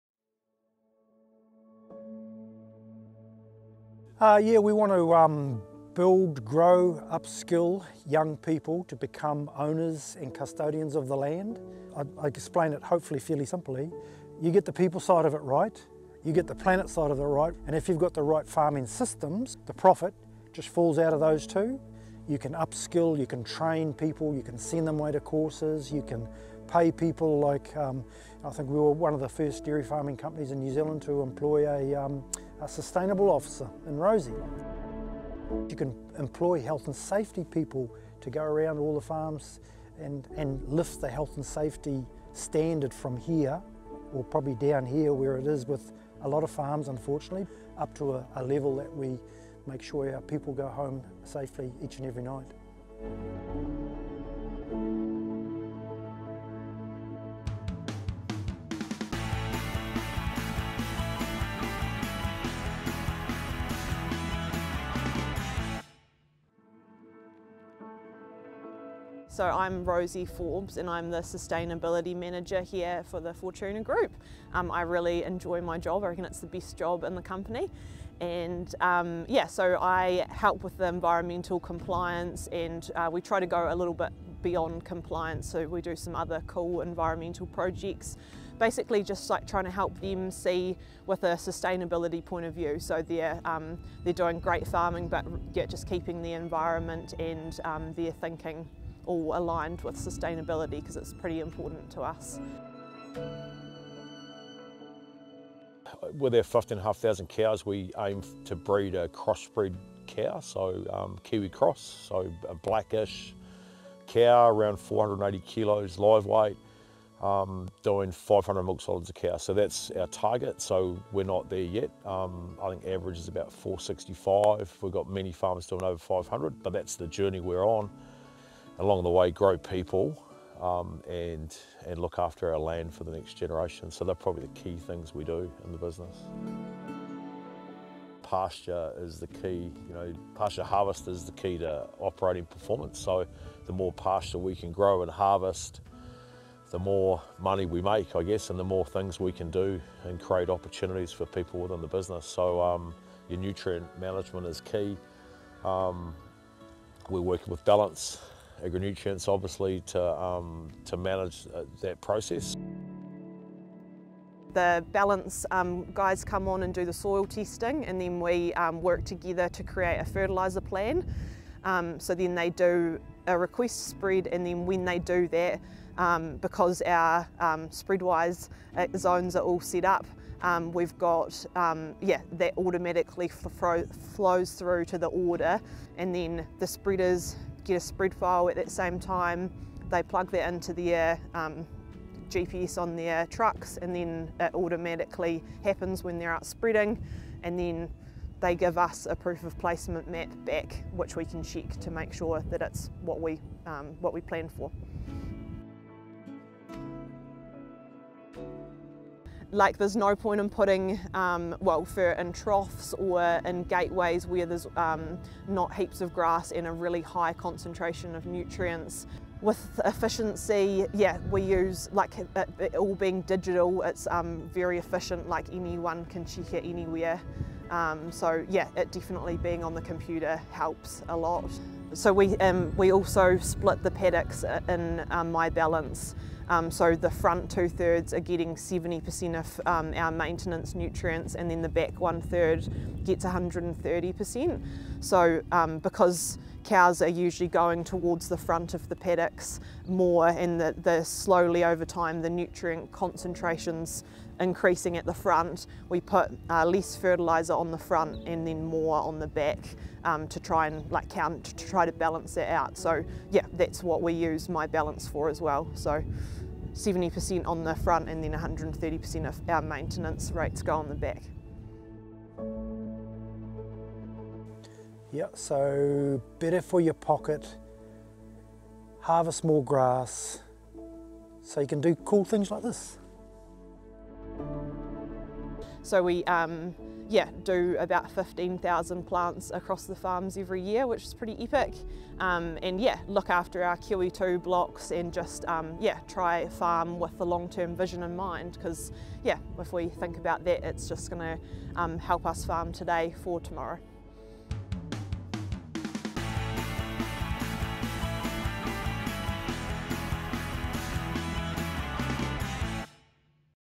Pulling no punches when it comes to environmental stewardship
Southland dairy farmer David Dodunski pulls no punches when it comes to environmental stewardship. To him, it’s not optional – it’s essential.

Dodunski is the founder of the Fortuna Group, comprising 23 dairy farms producing 7.6 million kgMS from 16,500 cows at peak milk. Using the latest science and technology to understand farm nutrient loads better, reducing waste and helping drive its sustainability journey is a core part of its farming operations.
The group also has 2800 effective hectares of support land, making it largely self-contained and in full control of youngstock development.

It did not start this big but began four decades ago, when Dodunski started sharemilking with just 31 cows. Fast-forward to today, and digital tools are at the heart of Fortuna’s nutrient management – particularly Ballance’s SpreadWise platform.

This episode was made possible with support from Ballance Agri-Nutrients.